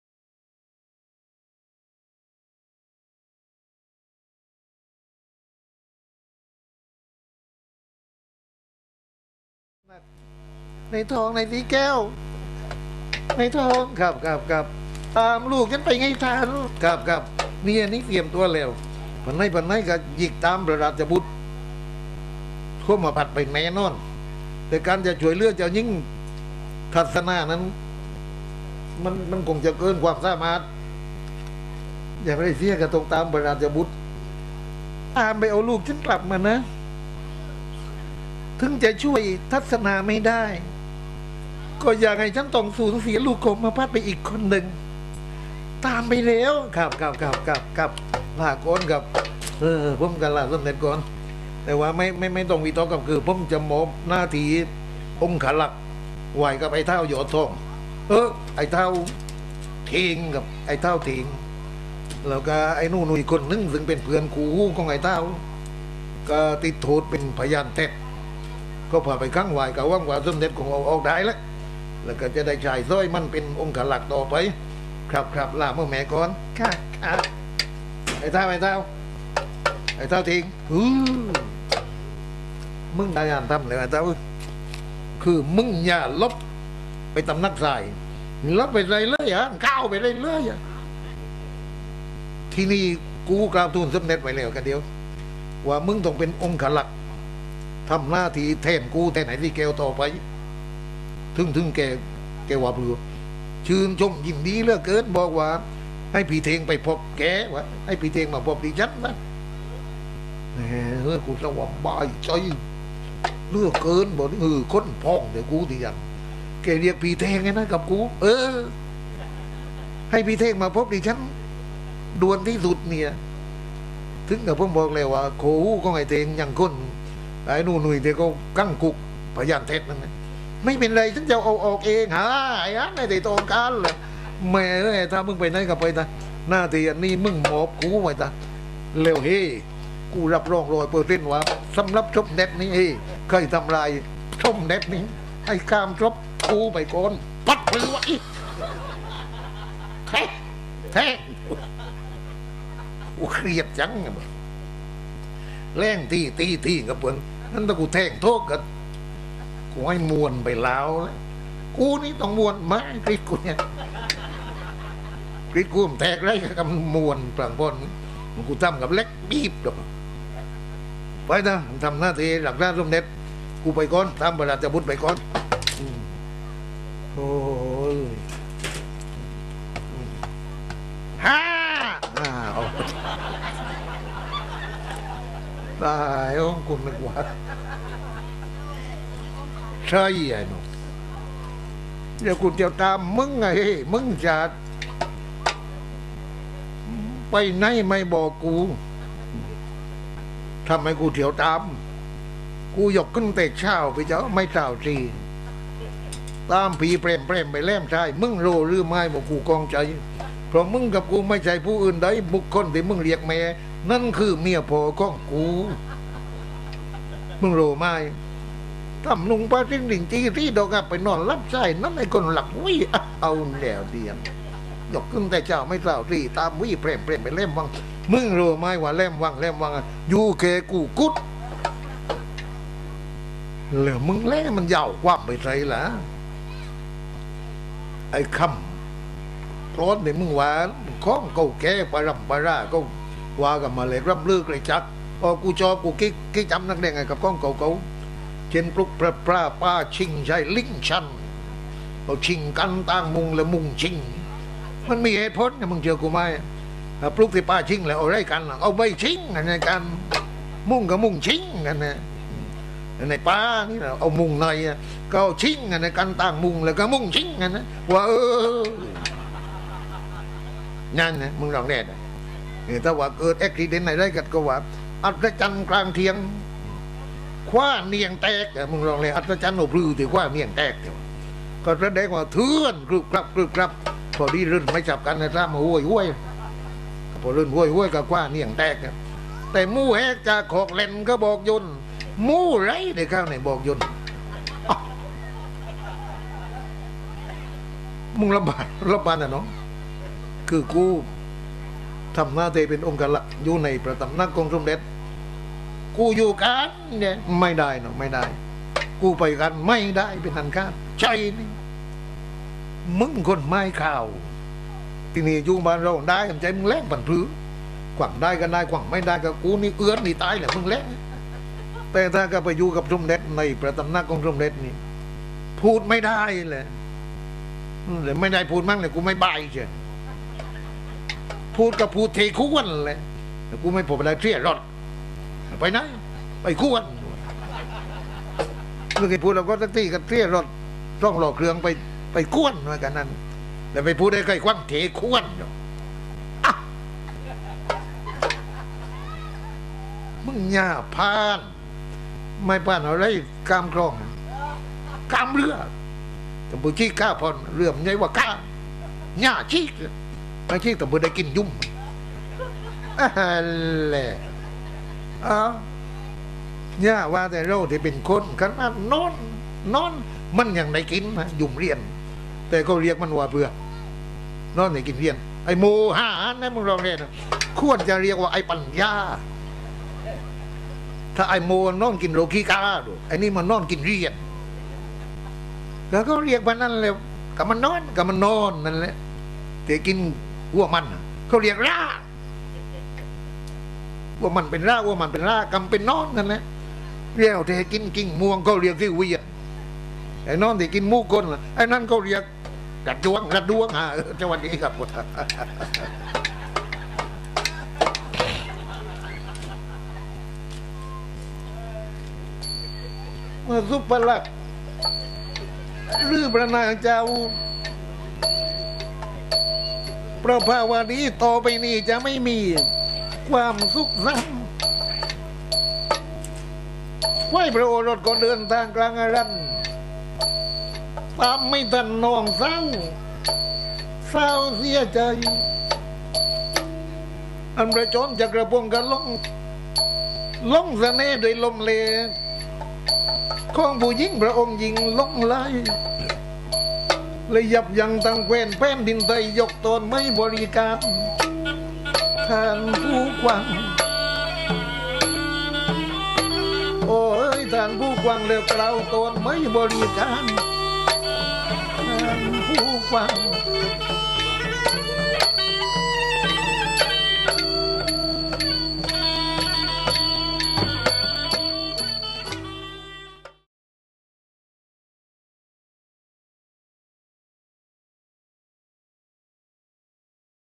หนังอรรถโฆษิต : เสน่ห์นาง : ตอนเท่งเป็นองครักษ์
หนังอรรถโฆษิต หนังตะลุงนามพระราชทาน (หนังฉิ้น ธรรมโฆษณ์)
ศิลปินแห่งชาติ สาขาศิลปะการแสดง (หนังตะลุง) ปี พ.ศ. ๒๕๓๒